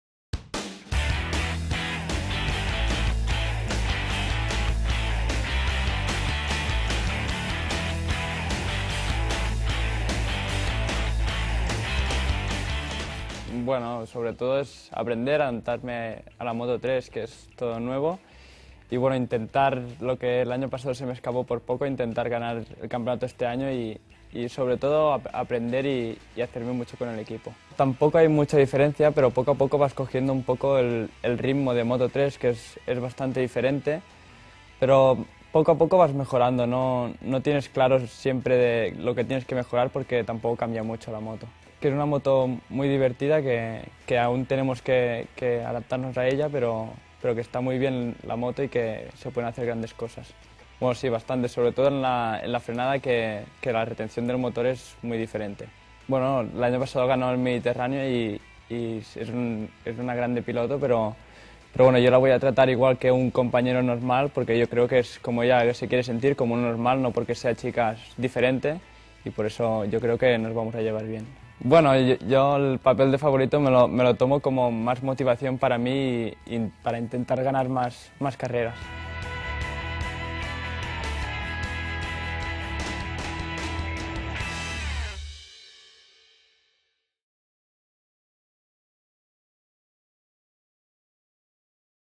Entrevista a Alex Márquez, piloto Repsol del CEV 2012
Entrevista exclusiva con Alex Márquez, piloto Repsol del Campeonato de España de Velocidad, subcampeón en la temporada 2011. Sigue a nuestro equipo en el CEV